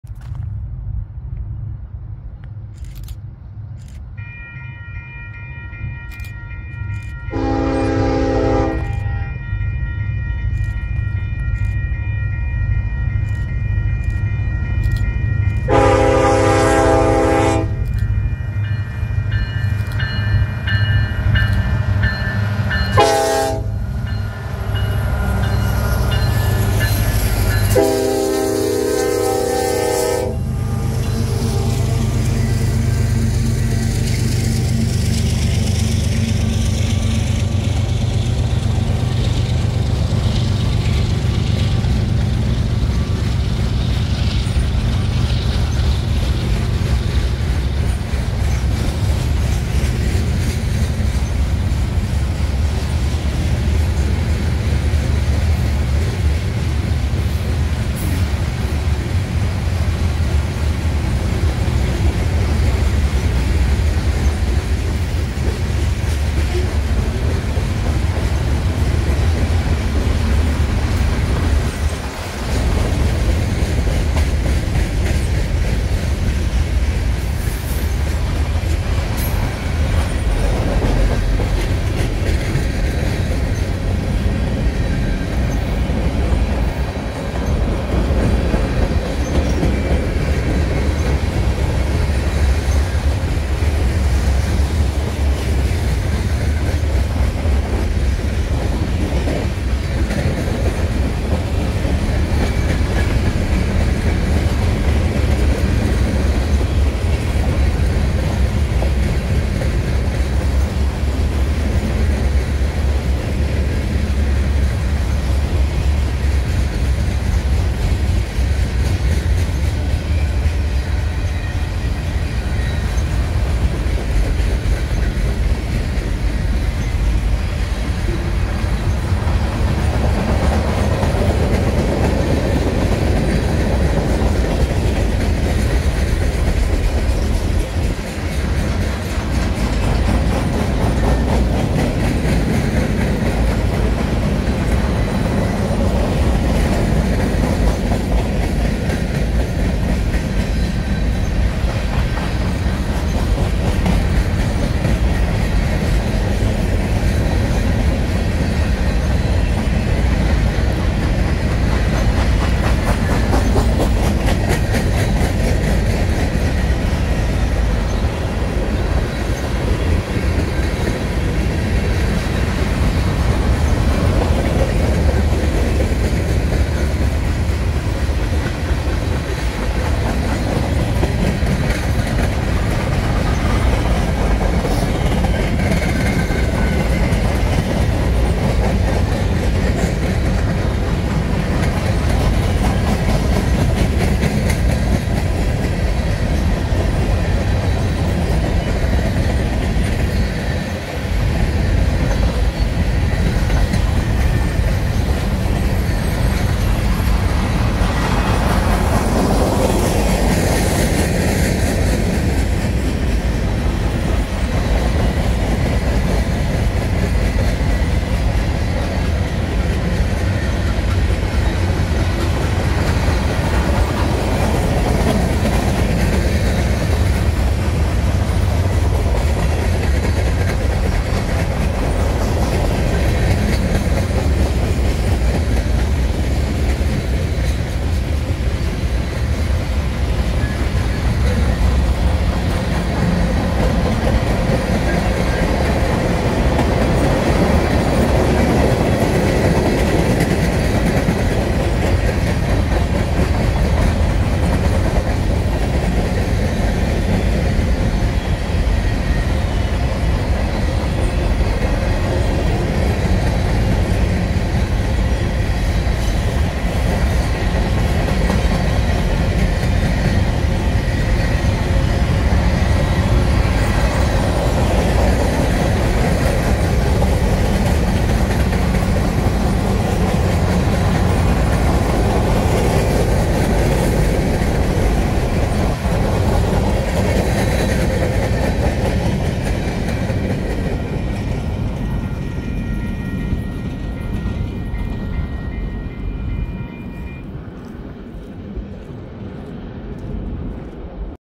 UP 8564 Leads a Manifest Train Off the Siding at Boardman, OR (Portland Subdivision)
All of the videos on this channel are the property of RailFan509™ and use of these without written consent from RailFan509™ is strictly prohibited